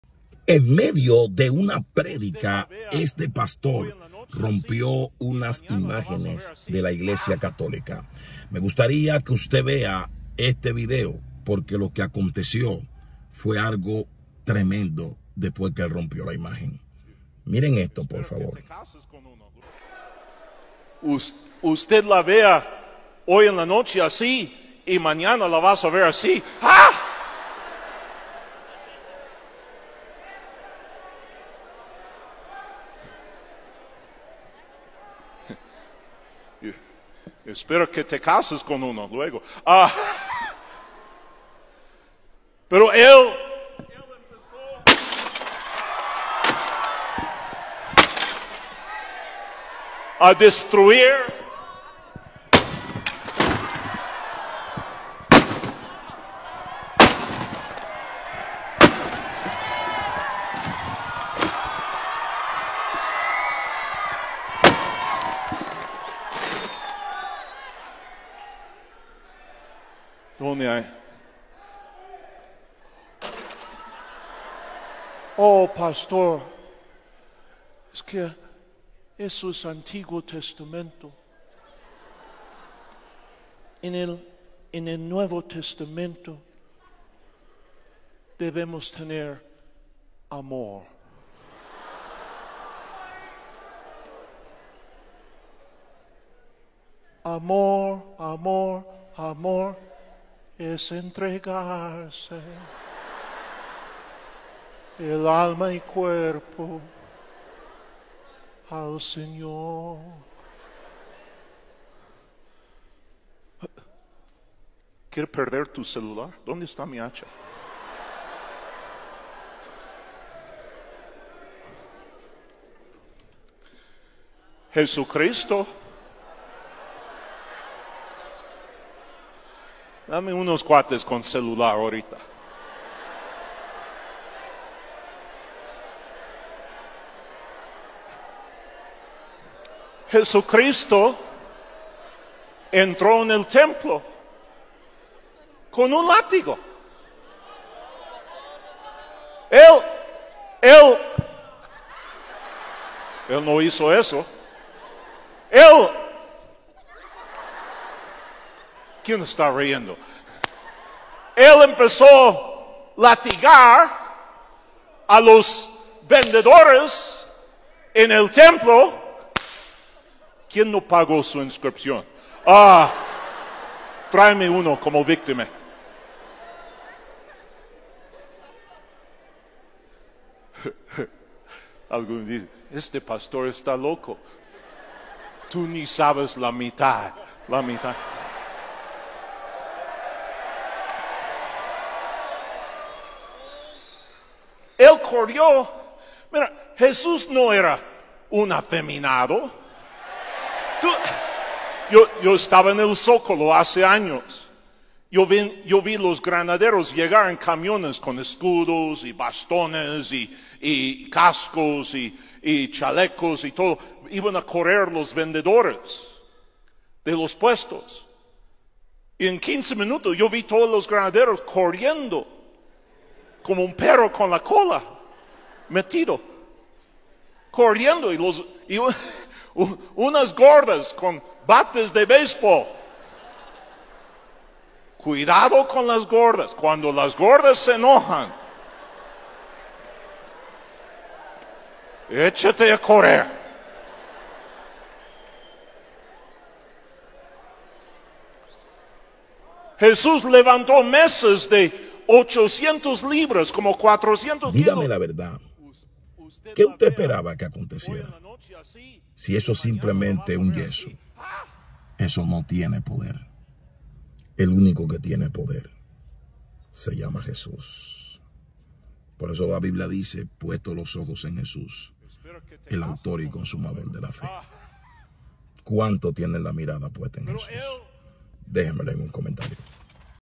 PASTOR ROMPE IMAGEN DE LA IGLESIA CATÓLICA MIREN LO QUE PASÓ 😳😳😳
Pastor Juan Gabriel peña rompen imagen de la iglesia católica y lo qué pasó después te impactará